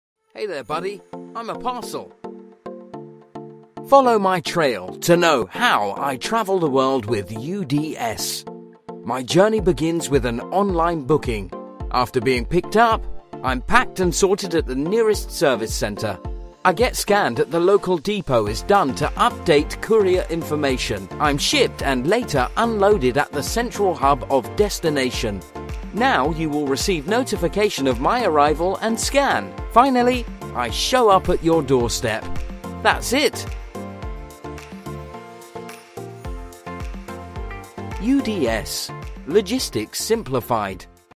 The Journey of a Parcel Delivery with UDS
Watch this video to know the journey of parcel delivery service in UK with UDS. or call: +44